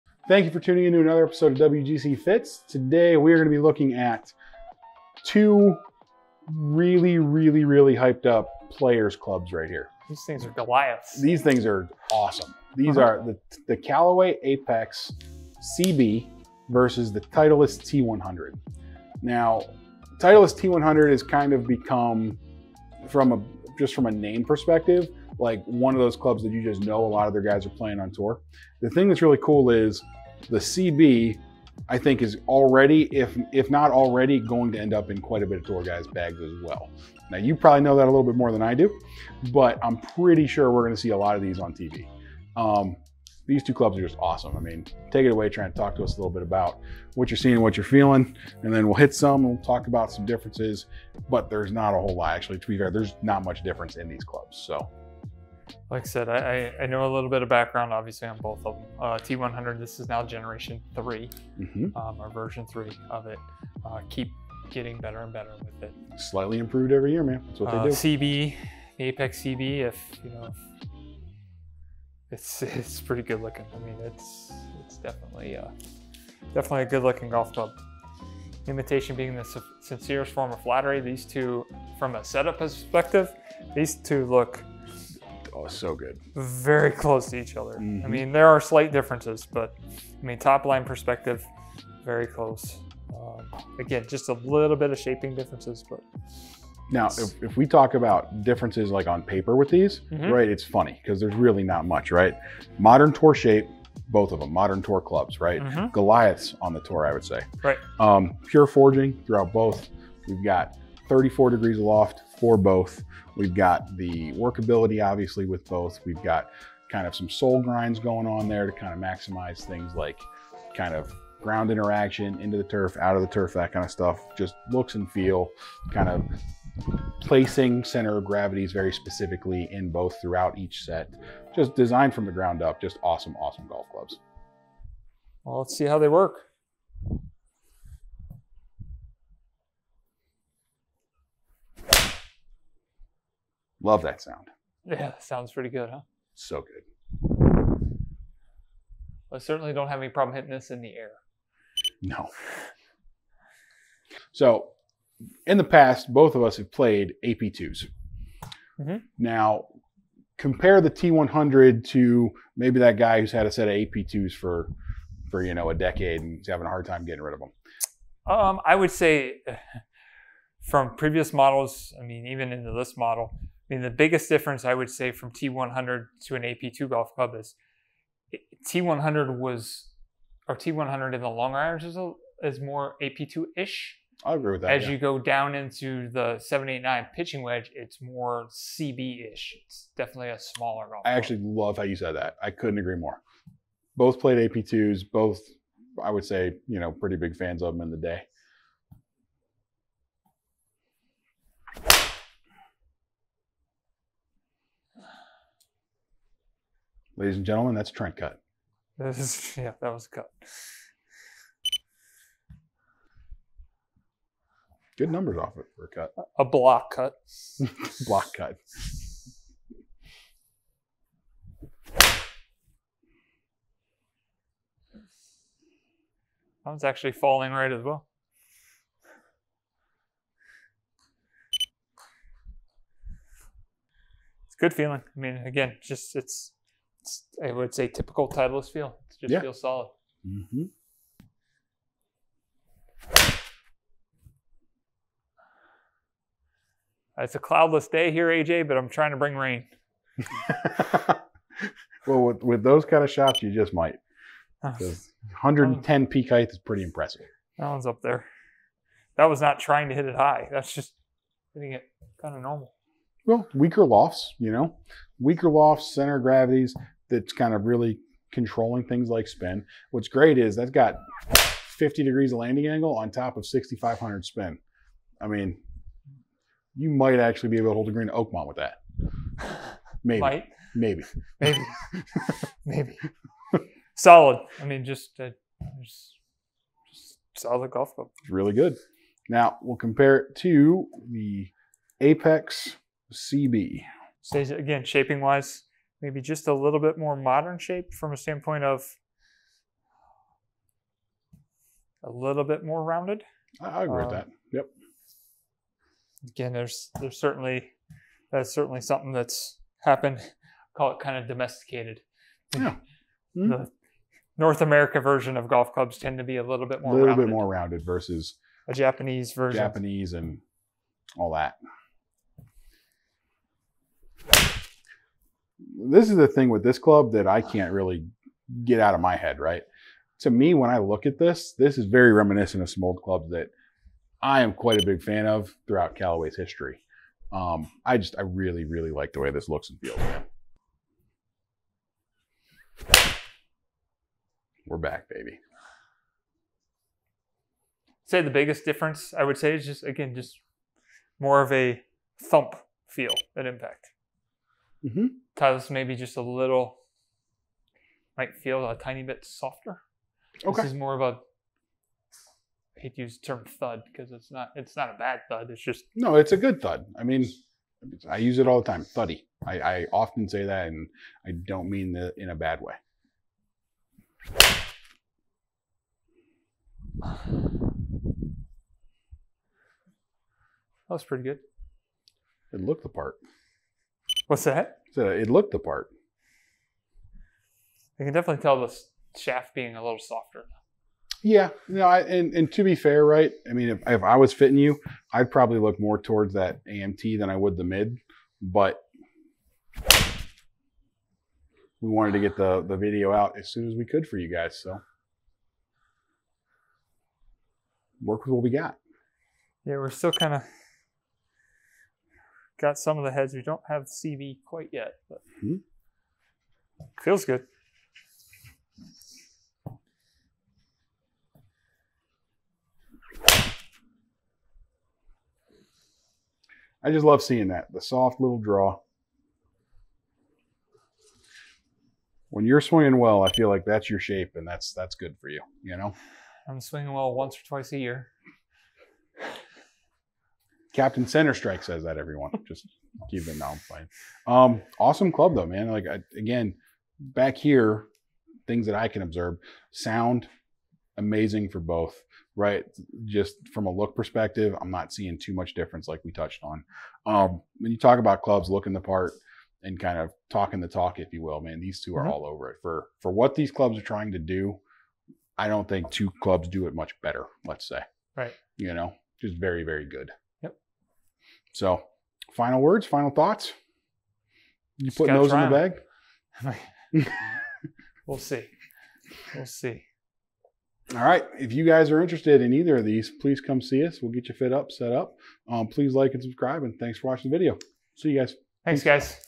Titleist T100 VS Callaway Apex CB | WGC Fits
We are putting two titans up against each other! Come on in to try them out for yourself!